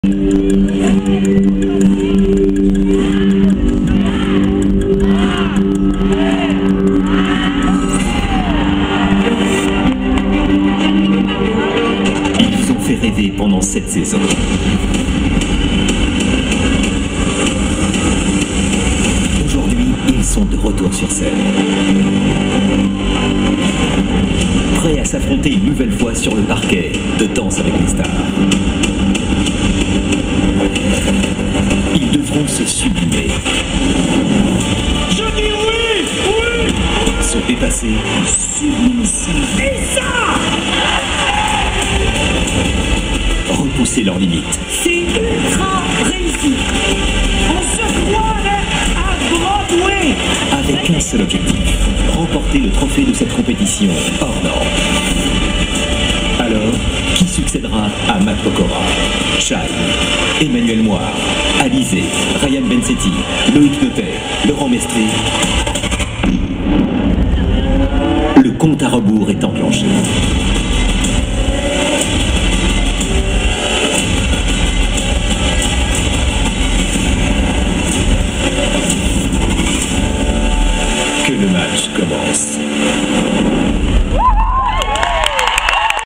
Début du Show - Danse Avec Les Stars 7 - Lyon - Hall Tony Garnier - HD (25/02/2017)
Voici le début du concert de la tournée de Danse Avec Les Stars Saison 7 à Lyon que c'était filmer par moi même le Samedi 25 Février 2016 à 15H00 à Lyon (Salle : Hall Tony Garnier)

cc : Alizée, Laurent Maistret, Karine Ferri, Florent Mothe et Sylvie Tellier